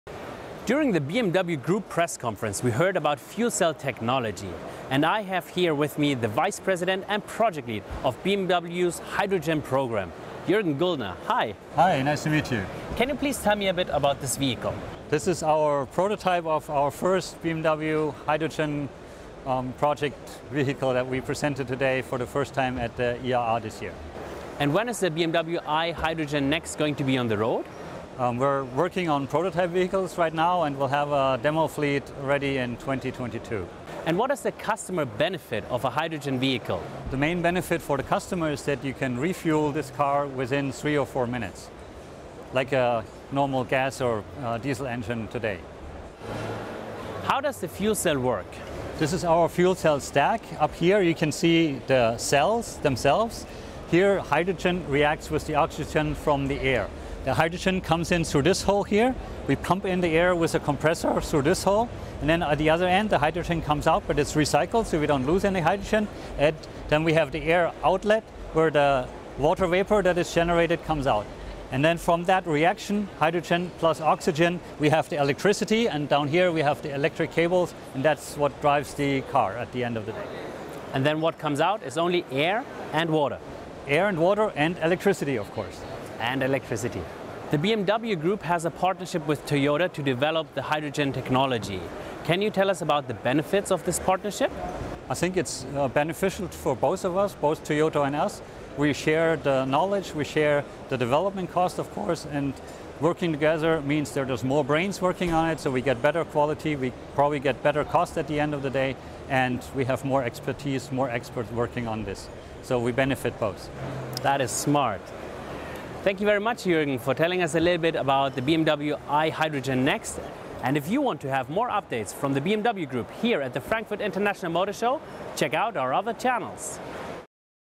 The BMW i Hydrogen NEXT at the IAA Cars 2019
Our Vice President for Hydrogen Fuel Cell Technology and Vehicles talks about the benefits of a hydrogen vehicle, and explains how a fuel cell works.

Our Hydrogen Project demo fleet will be ready by 2022. And like a combustion engine today, they can be refuelled in just 3-4 minutes.